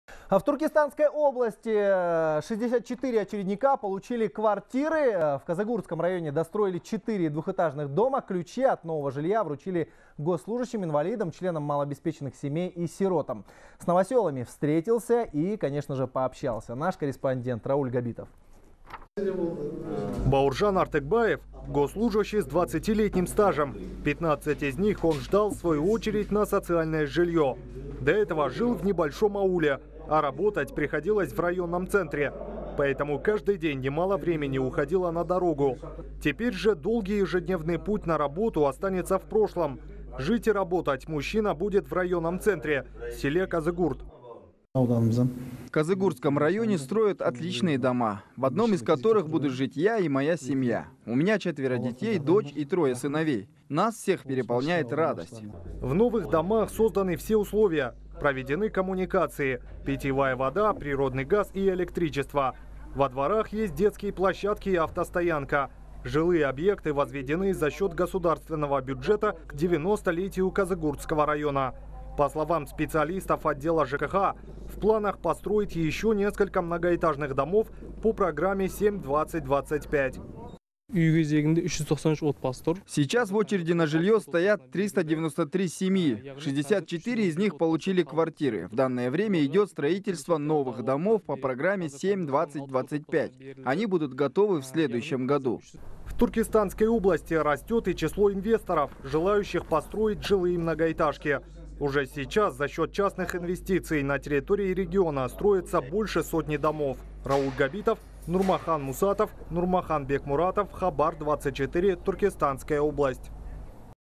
64 очередника получили квартиры в Туркестанской области
В Казыгуртском районе достроили 4 двухэтажных дома. Ключи от нового жилья вручили госслужащим, инвалидам, членам малообеспеченных семей и сиротам, сообщает корреспондент «Хабар 24».

Бауыржан Артыкбаев – госслужащий с 20-летним стажем, 15 из них он ждал своей очереди на социальное жилье. До этого жил в небольшом ауле, а работать приходилось в районном центре. Поэтому каждый день немало времени уходило на дорогу. Теперь же долгий ежедневный путь на работу останется в прошлом. Жить и работать мужчина будет в районном центре – селе Казыгурт.